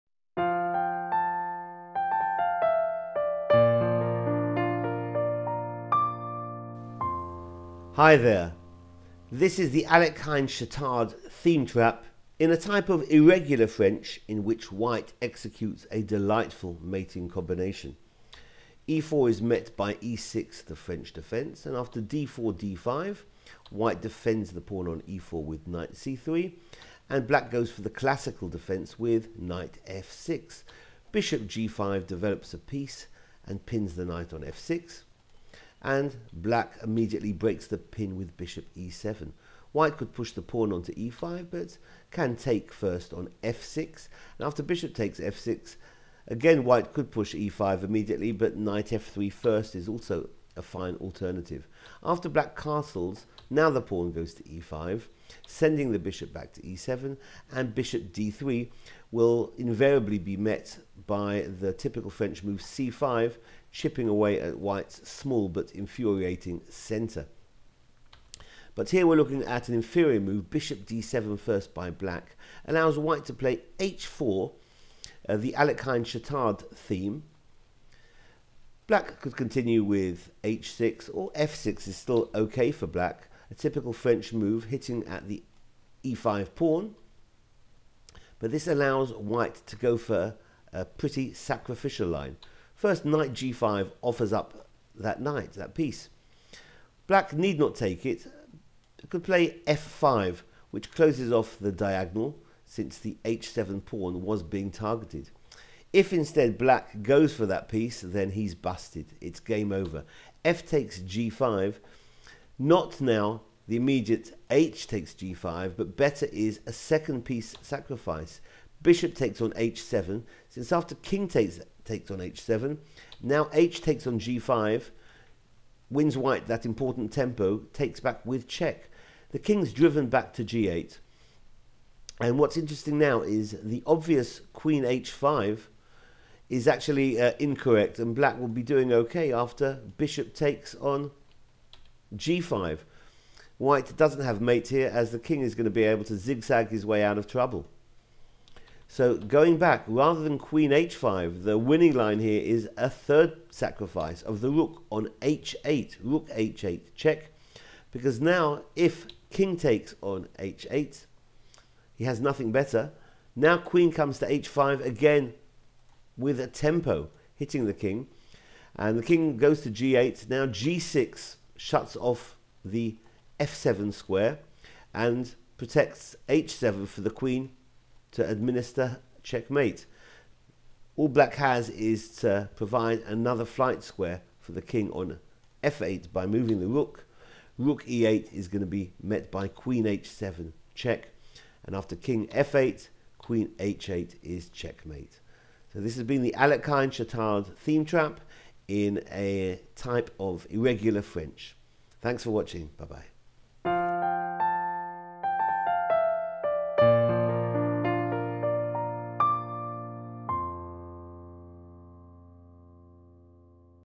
French - Alekhine Chatard Theme Trap (trap no. 579)
1.e4 e6 2.d4 d5 3.Nc3 Nf6 4.Bg5 Be7 5.Bxf6 Bxf6 6.Nf3 0-0 7.e5 Be7 8.Bd3 Bd7  9.h4 f6 10.Ng5 fxg5 11.Bxh7+ Kxh7 12.hxg5+ Kg8 13.Rh8+ Kxh8 14.Qh5+ Kg8 15.g6 Re8 16.Qh7+ Kf8 17.Qh8#